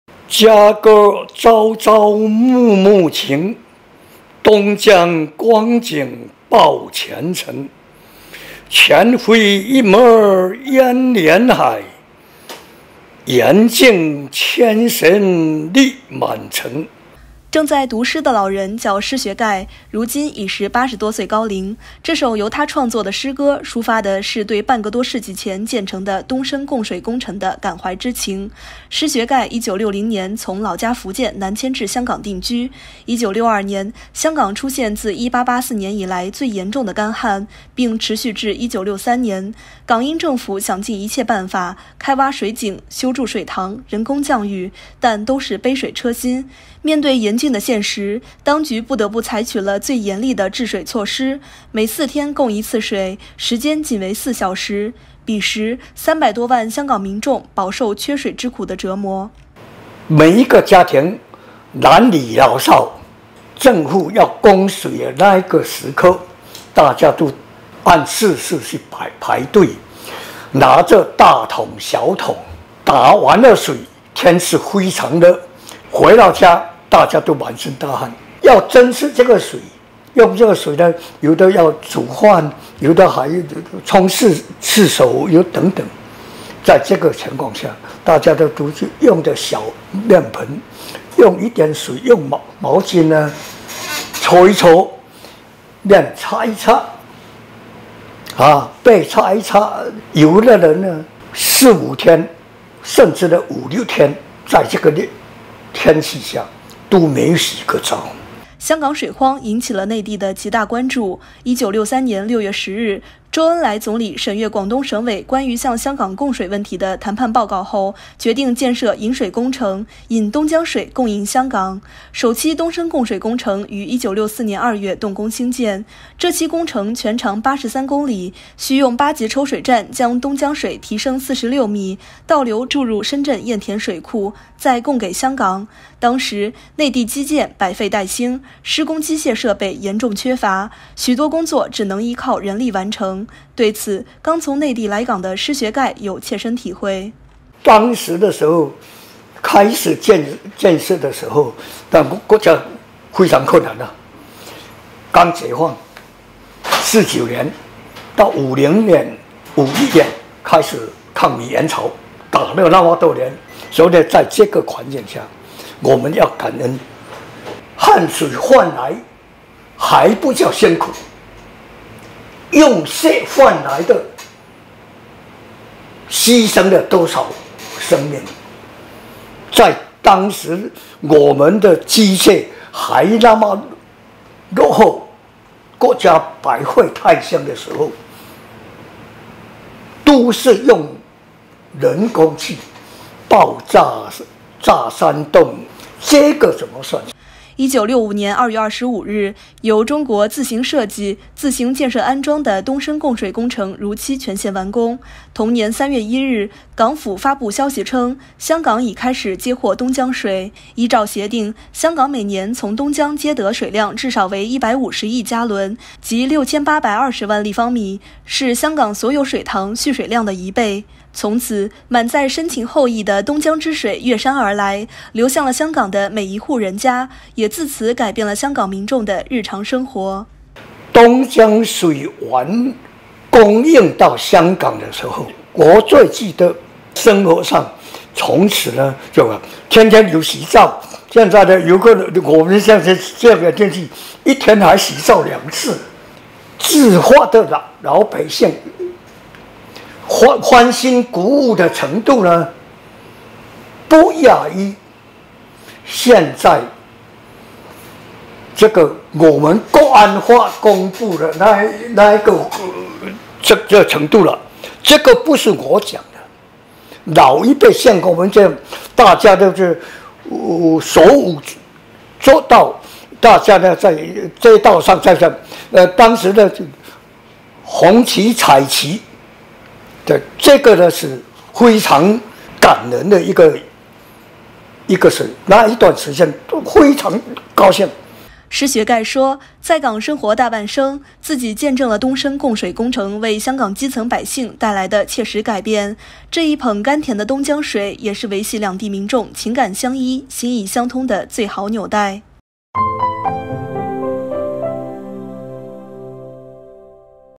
港翁追忆东深供水情：从“五六天洗一次澡”到“一天冲两次凉”
正在读诗的老人叫施学概，如今已是80多岁高龄，这首由他创作的诗歌抒发的是对半个多世纪前建成的东深供水工程的感怀之情。施学概1960年从老家福建南迁至香港定居。1962年，香港出现自1884年以来最严重的干旱，并持续至1963年。港英政府想尽一切办法，开挖水井、修筑水塘、人工降雨，但都是杯水车薪。面对严峻的现实，当局不得不采取了严厉的制水措施，每4天供一次水，时间仅为4小时。彼时，300多万香港民众饱受缺水之苦的折磨。